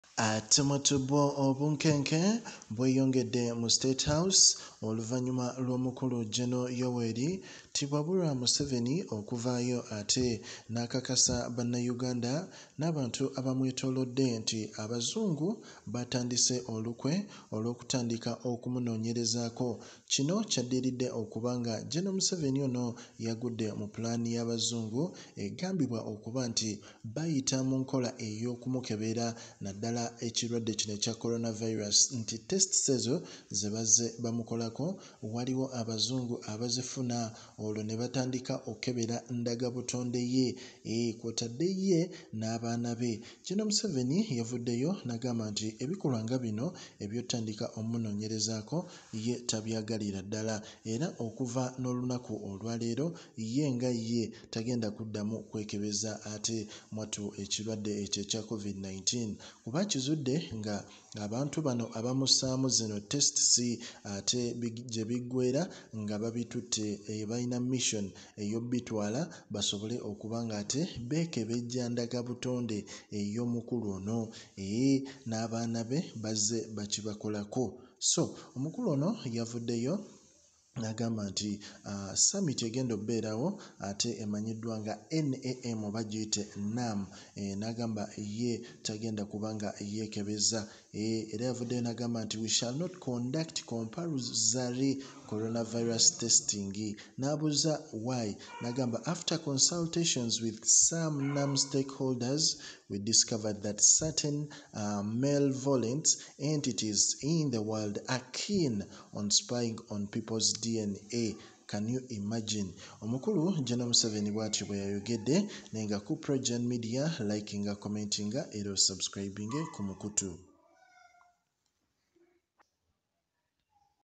Obunkenke ewa museveni Abazungu Bayside Mu DNA ofuna ebimukwatako Bwiino omukambwe azzuuse
Museveni accuses Kampala based embassies of spying on him, his children
Kampala, Uganda | By Michael Wandati | In a social media statement, President Museveni has claimed that certain foreign missions based in Kampala are actively spying on him, seeking his DNA and that of his children for undisclosed motives.

Mr Museveni, dismissing these intentions as superficial and unserious, highlighted the issue in a recent communication.

Consequently, the President has issued a decree stating that there will be no mandatory COVID-19 testing during the NAM summit to prevent any potential espionage on DNA.

Addressing the issue, he stated, “During the NAM summit, we shall not conduct compulsory coronavirus testing. Why? After consultations with some NAM stakeholders, we discovered that certain malevolent entities in the world are keen on spying on people’s DNA. Can you imagine!”

Expressing his astonishment, he added, “Indeed, some patriots of Uganda were most surprised when certain embassies in Kampala were actively seeking my DNA, as well as that of my children. How shallow and unserious!.”

Earlier this year, African Presidents, particularly those perceived as pro-Russian and attendees of the Russia-Africa summit in St Petersburg, experienced heightened security measures. This response came after leaked intelligence suggested potential assassination plans originating from Western sources.

According to the Kremlin, there is a select group that has been hired by Britain to act on leaders in Africa.